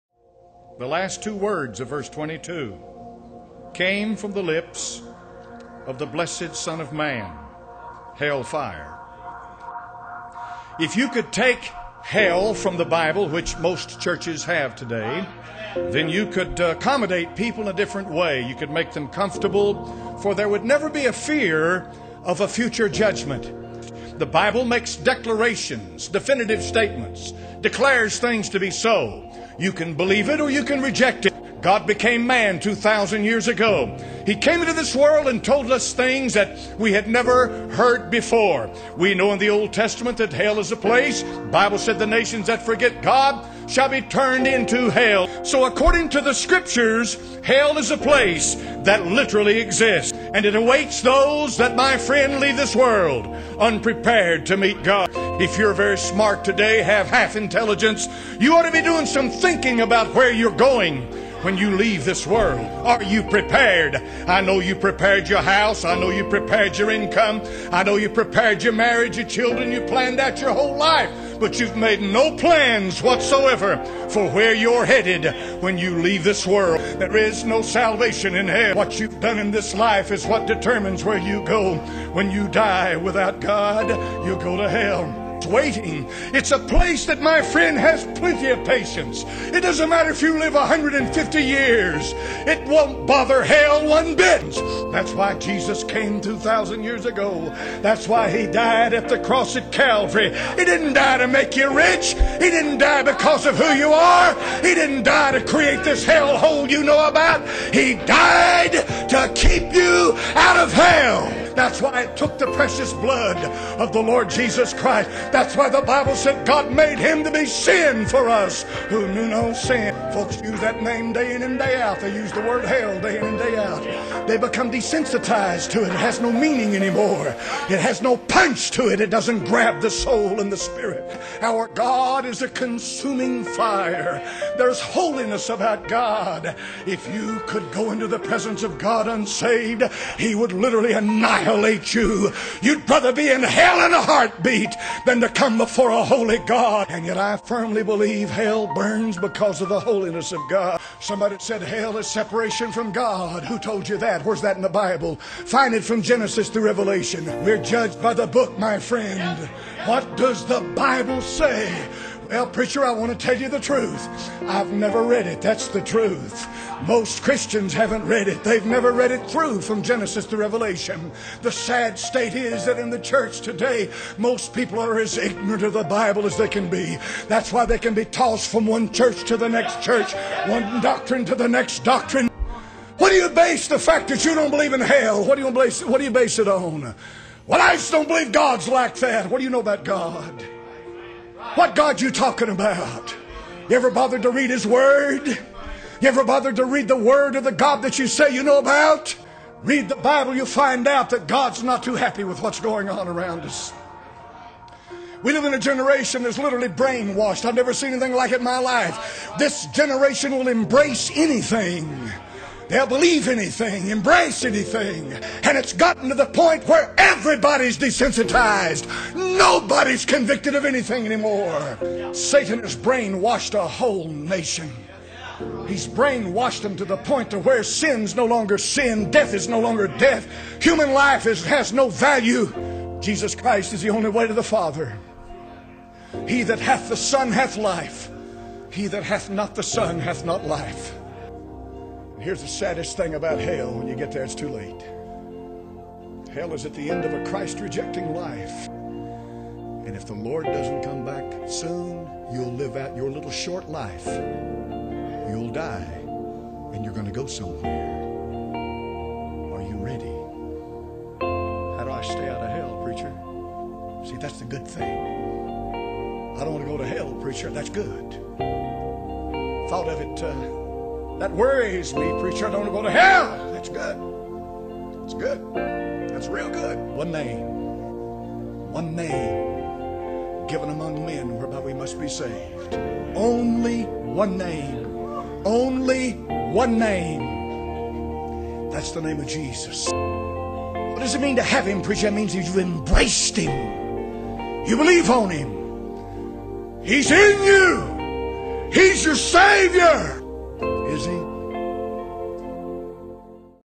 Hell Fire - Charles Lawson
Based off Matthew 5:22, Charles tells us what hell is like.  I do not own the copy rights to the music.